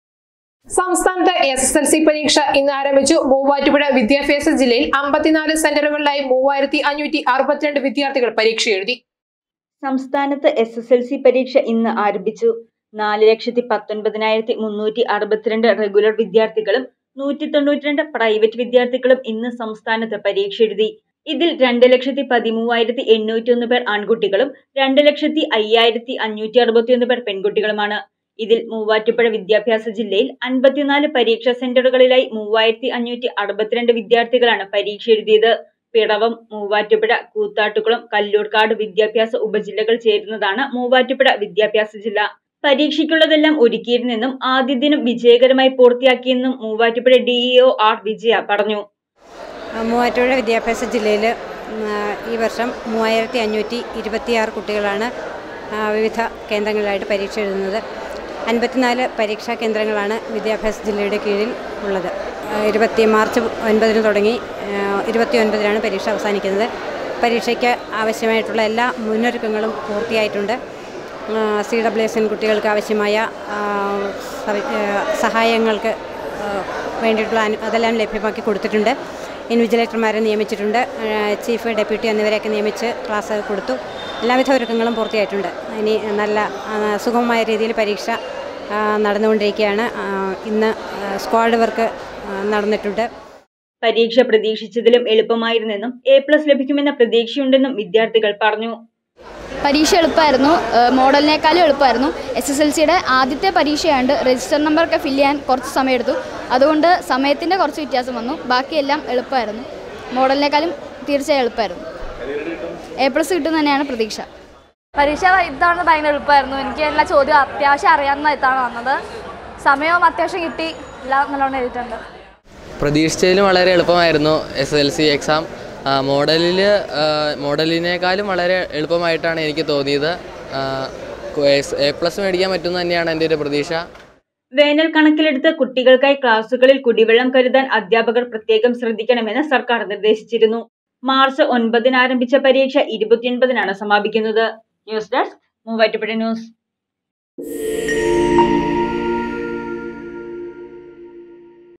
SSLC പരീക്ഷ ആരംഭിച്ചു ; മൂവാറ്റുപുഴയിൽ 3562 വിദ്യാര്‍ത്ഥികളാണ് പരീക്ഷ എഴുതിയത്...
Muvattupuzha News